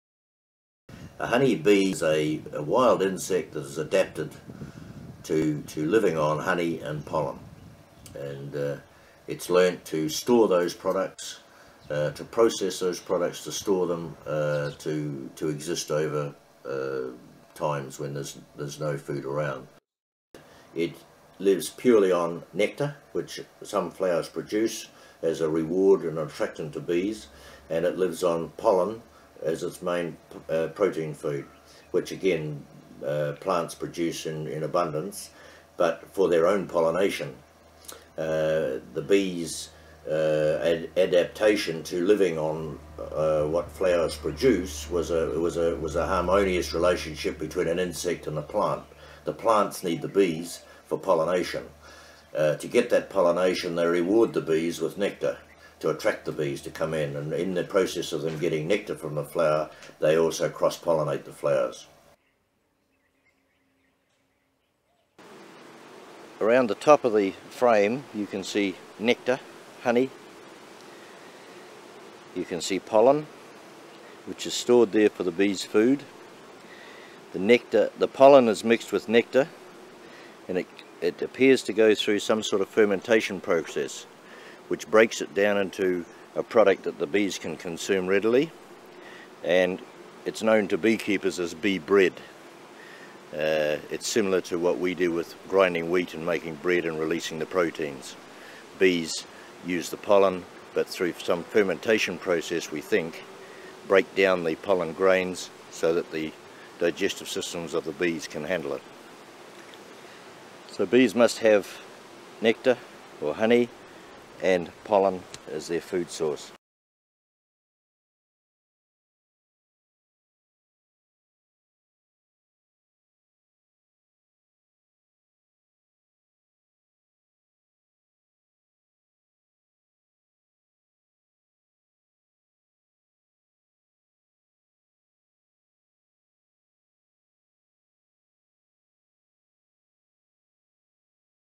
Part 2 - Bees & Flowers - Guide to Beekeeping in the Tropics with Gavin McKenzie
Beekeeping in Fiji

'A Guide to Beekeeping in the Tropics’ with Gavin McKenzie

'A Guide to Beekeeping in the Tropics’ is comprised of three DVDs. On each DVD, Gavin McKenzie of McKenzie Beelines in Fiji will be taking you through the many important aspects of beekeeping in the tropics.

Mr. McKenzie has over 50 years of experience in beekeeping, having worked in Papua New Guinea, New Zealand and Fiji.

While this project directly concerns beekeeping in Fiji, the information given is applicable to all tropical beekeepers throughout the wider Asia Pacific.